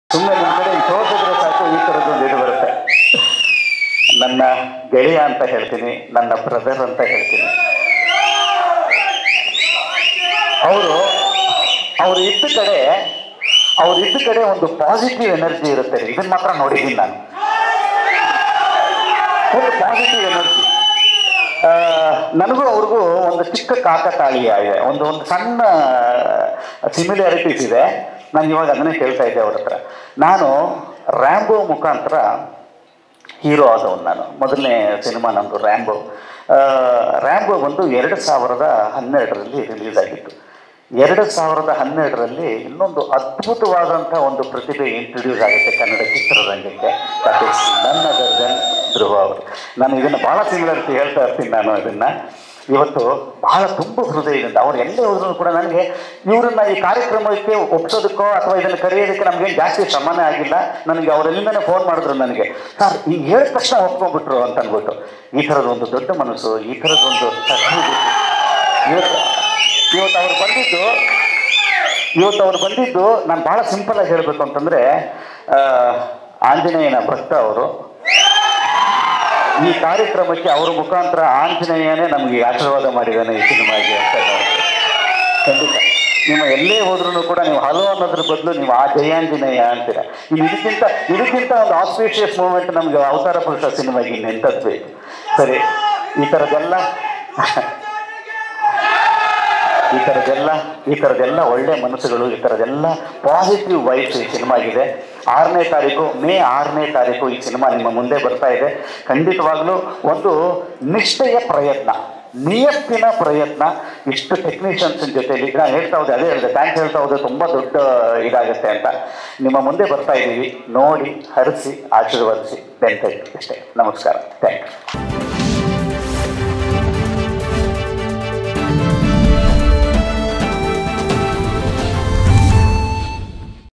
Dhruva Sarja : ಧ್ರುವ ಒಪ್ಸೋಕೆ ಜಾಸ್ತಿ ಟೈಮ್ ತಗೊಳಿಲ್ಲ! Sharan | Ashika Ranghnath | Avatara Purusha
News Channel  Launching Soon...